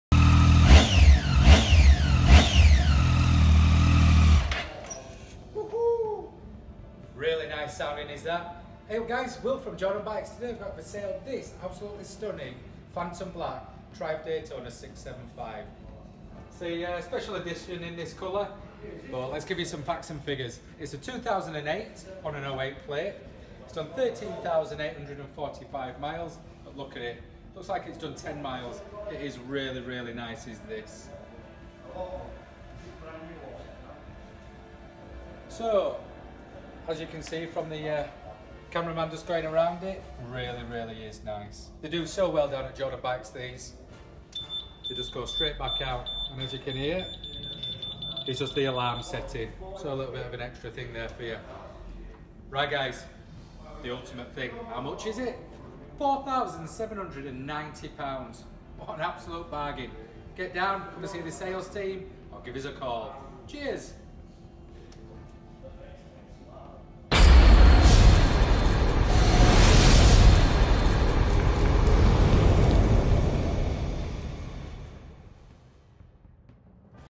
JORDAN BIKES - FOR SALE TRIUMPH DAYTONA 675 Special Edition 2008/08 13845 miles £4790
FOR SALE - Triumph Daytona 675 Special Edition Phantom Black. 2008/08 plate, this immaculate 2 owner bike has done just 13,845 miles from new with a thorough service history. It looks stunning in the flesh, it just needs some John Player Special decals on it ! It's all yours for just £4790